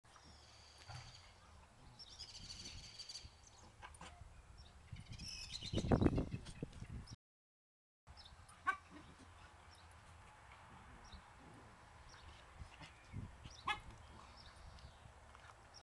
Poor Lucas.AVI
This is a dog with true patience, my poor German Shepherd Lucas just wants to play with his ball in peace, Lula has other ideas, he never loses his temper though, even when shes hanging off his face, little minx.